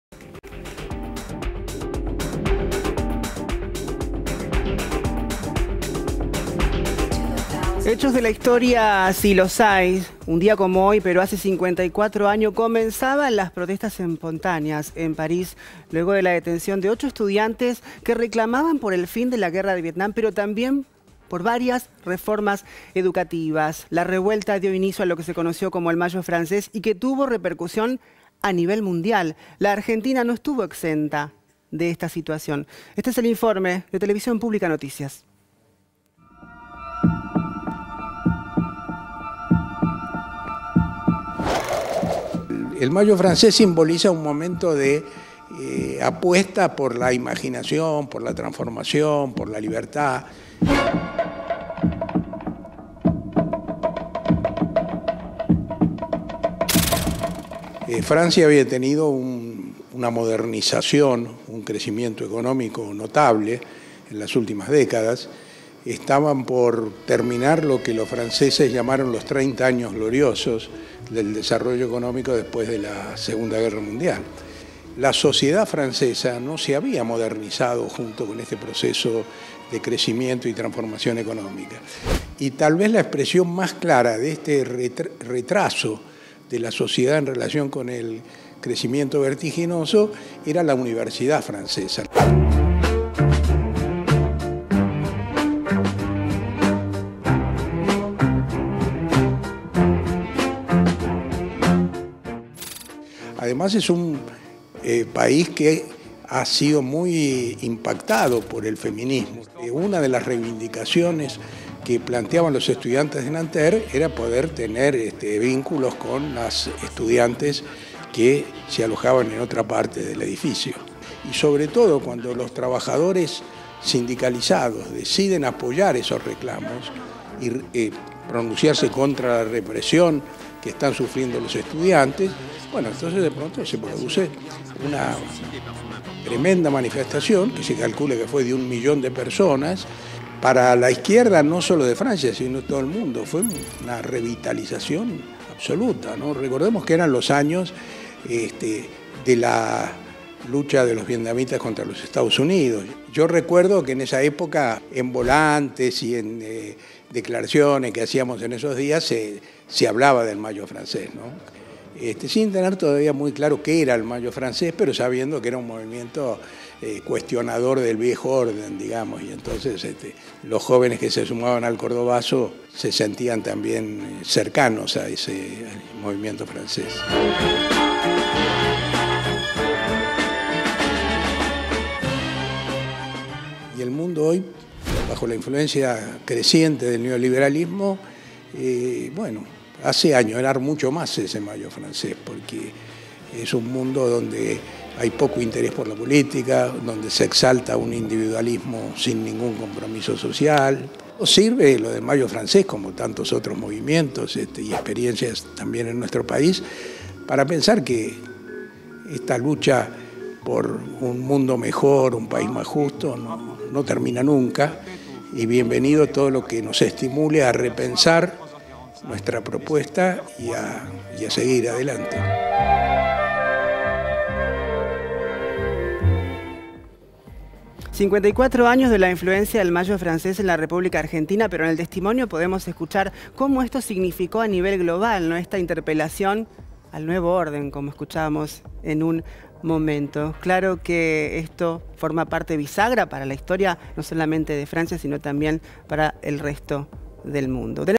La influencia del mayo francés en la Argentina
Hace 54 años de desarrollaban manifestaciones en las calles de París, dando lugar a lo que se conocería como el "mayo francés". Dialogamos con Eduardo Jozami, militante político y de DDHH, en torno al impacto de dichas protestas en la sociedad argentina.